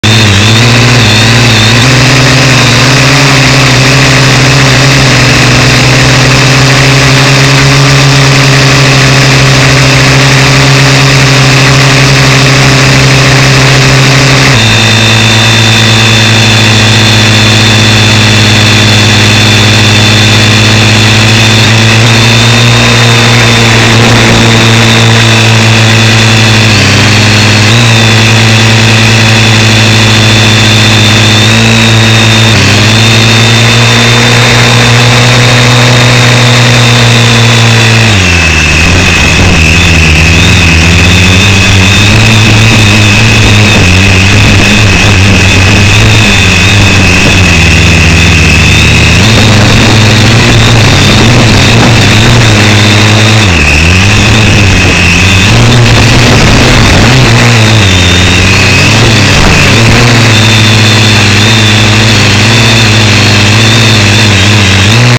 Höhenrekord (?) mit der AR.Drone
gefilmt mit der Guncam bei totaler Windstille über der autofreien Siedlung Weißenburg. Der orangene Herr am Boden ist der Pilot.